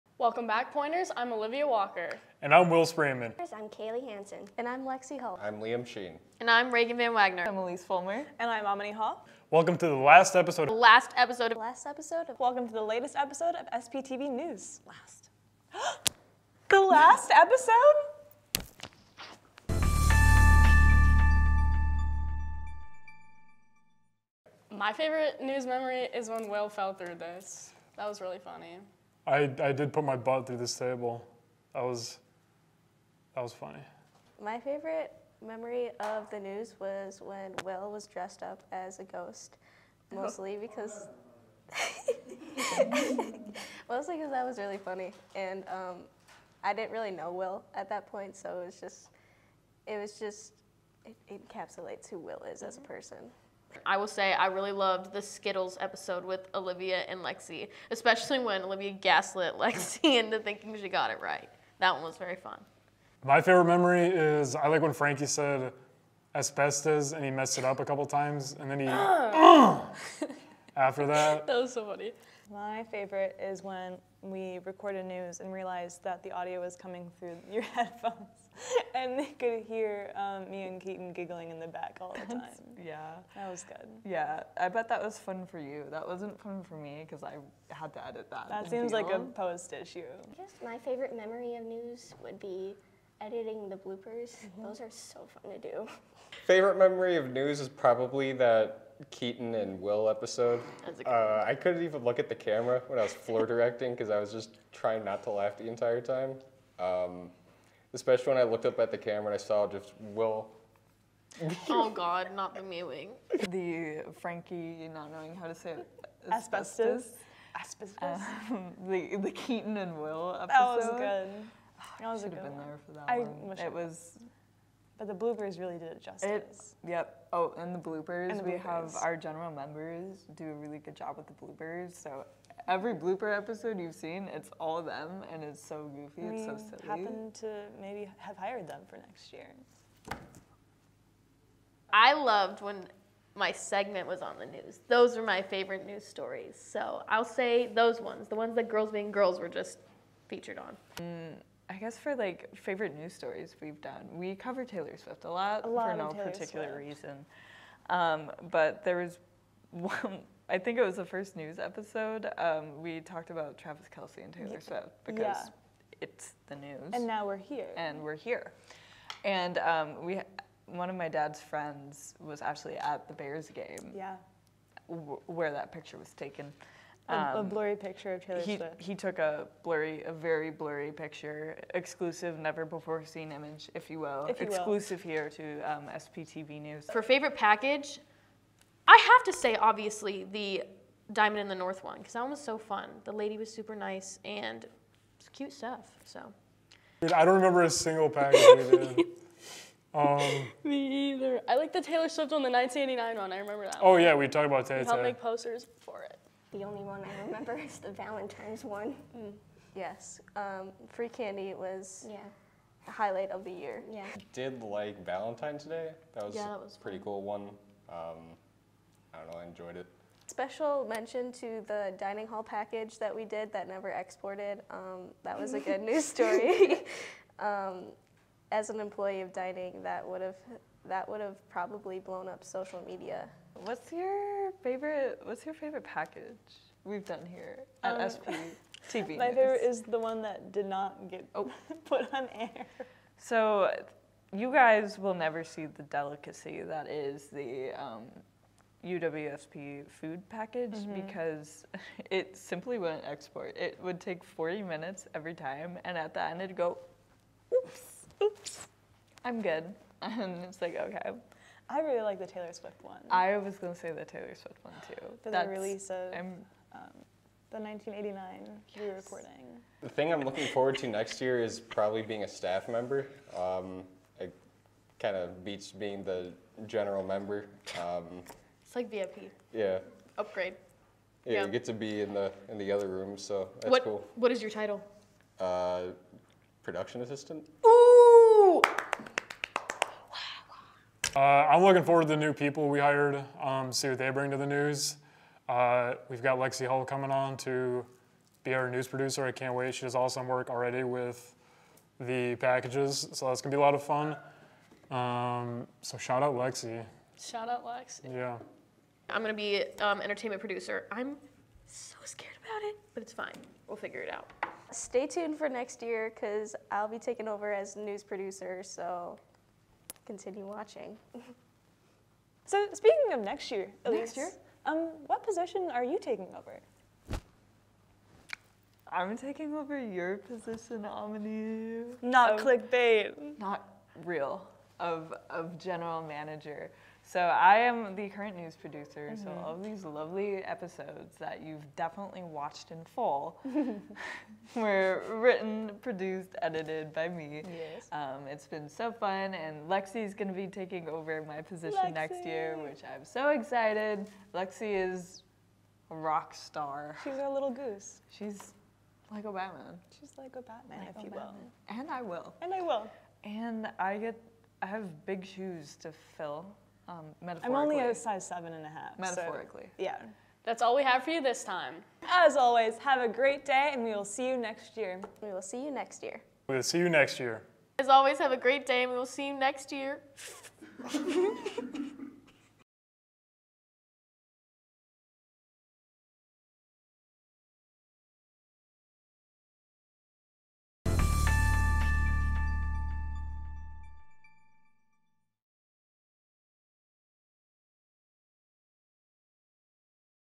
SPTV: LAST EPISODE OF THE SEMESTER!!
Welcome to the last episode of SPTV News of Spring 2024!!
Special Thanks to the general members and staff of Pointer Studios!!
We cant wait to see you next year :)

Anchors:
Olivia Walker
Will Spreeman
Kayli Hansen
Lexi Hull
Liam Sheen
Raegan VanWagner
Aminee Hoff
Elise Fulmer

Edited by:
Elise Fulmer
Lexi Hull